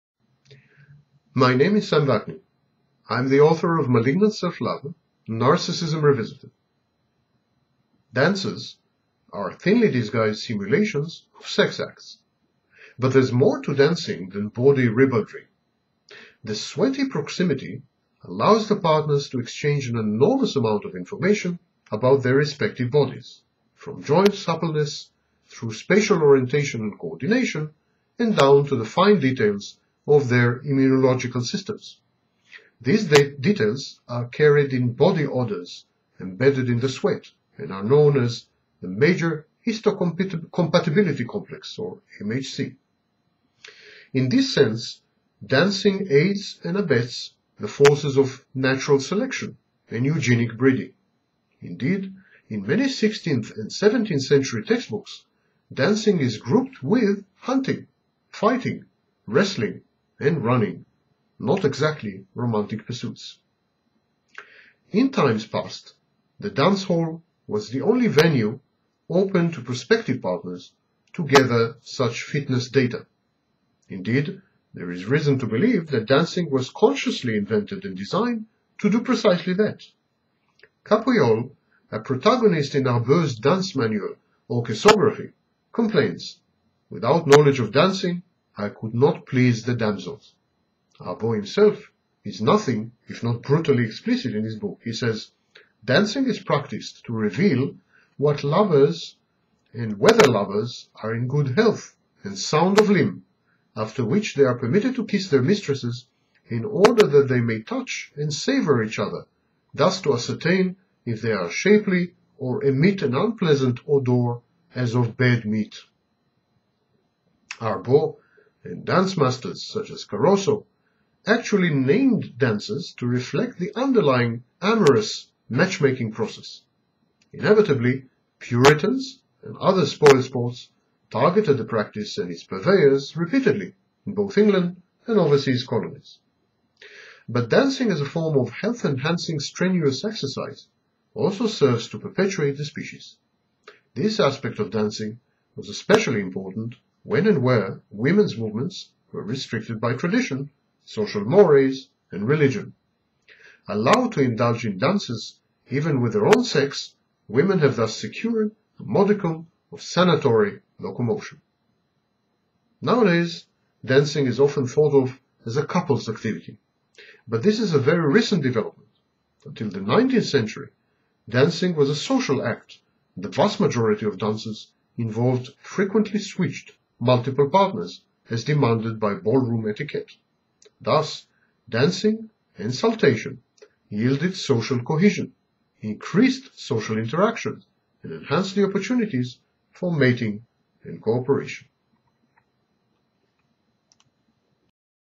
Dancing as Evolutionary Strategy
Dances are thinly disguised simulations of sex acts. But there's more to dancing than bawdy ribaldry. The sweaty proximity allows the partners to exchange an enormous amount of information about their respective bodies: from joint suppleness, through spatial orientation and coordination, and down to the fine details of their immunological systems (such as the major histocompatibility complex MHC) carried by their body odours. In this sense, dancing aids and abets the forces of natural selection and eugenic breeding. Indeed, in many 16th and 17th century textbooks dancing is grouped with hunting, fighting, wrestling, and running.